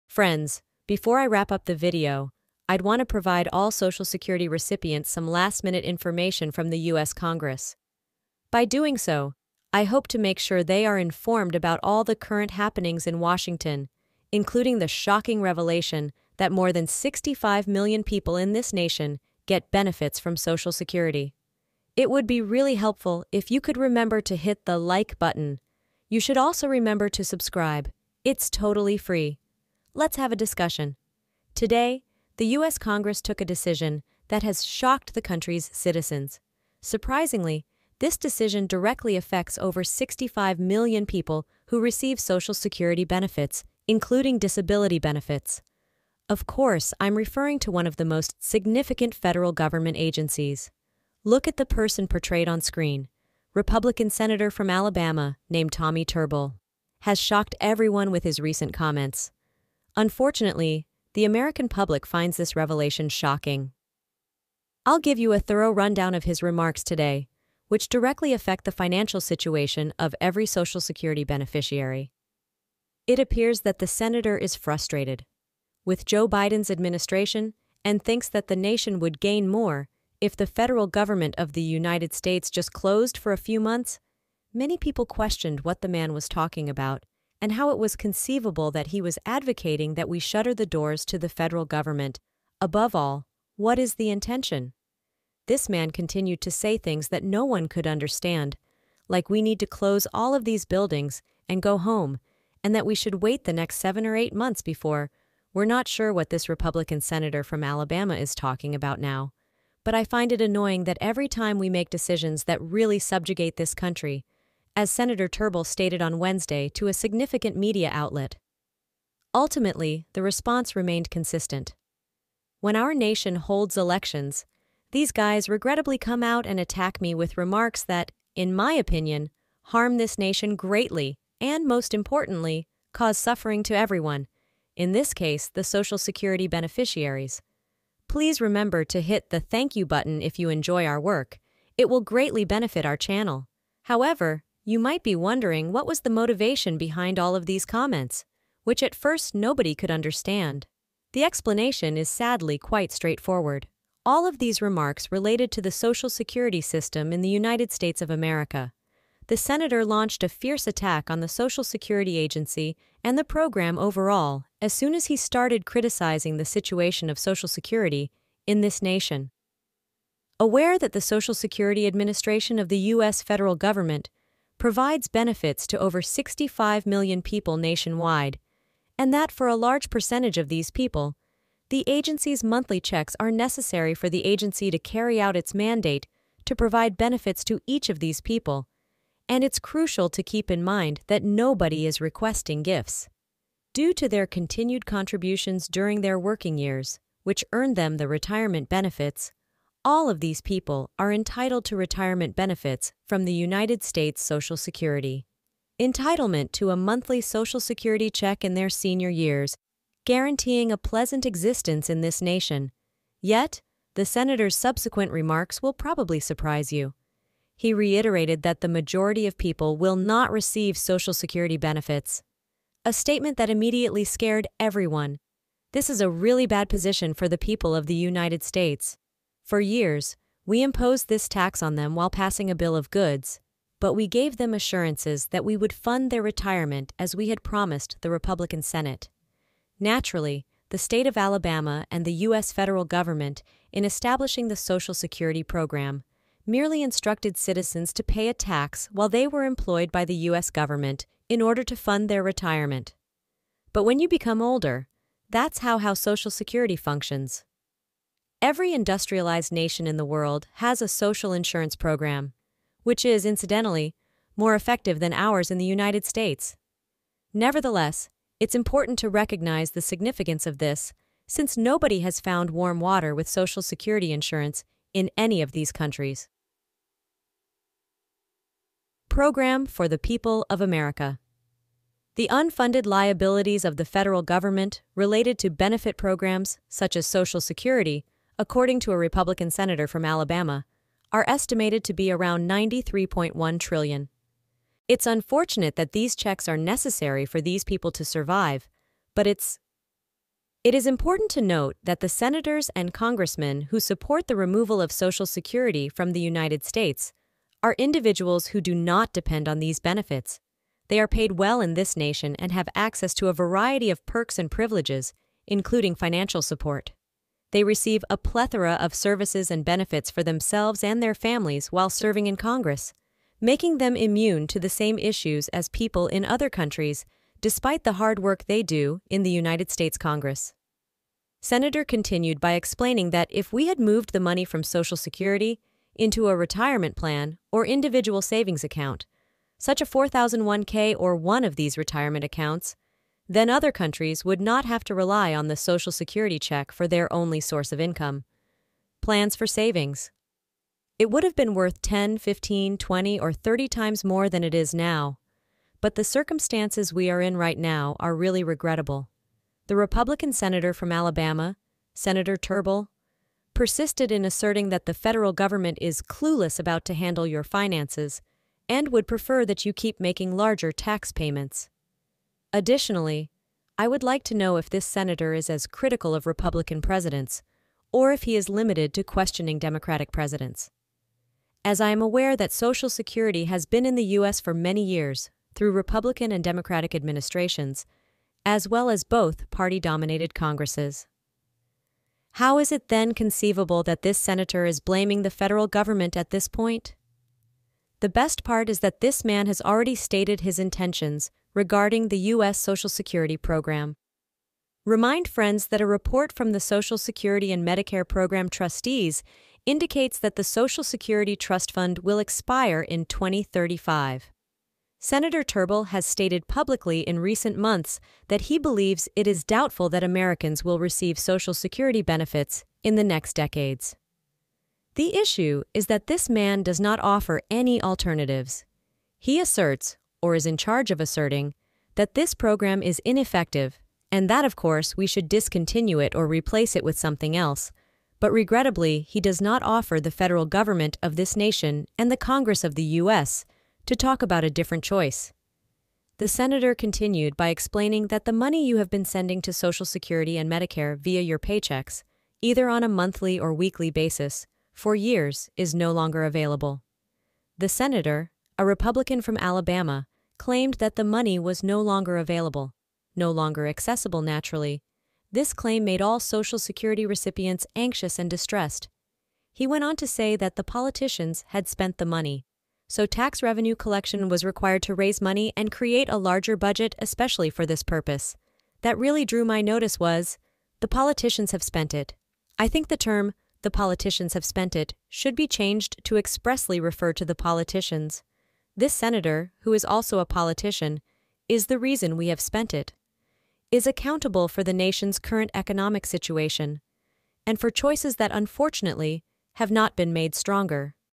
Surprise Boost: Social Security Increase for 65 Million Beneficiaries on May 27th! - SS, SSI, SSDI
Before I finish the video, I want to share some important news from the US Congress with Social Security recipients. Over 65 million people in the country get benefits from Social Security. Today, a decision from the US Congress shocked everyone, especially the 65 million Social Security beneficiaries. Senator Tommy Turbul from Alabama made comments criticizing the government, suggesting it should shut down for a few months. This confused many people. He also questioned the Social Security system, which supports millions of Americans. He thinks the money for Social Security is gone, and he doesn't offer any solutions. This is worrying because many rely on Social Security for retirement. It's frustrating that politicians like him have mishandled the situation, leaving people anxious about their future benefits.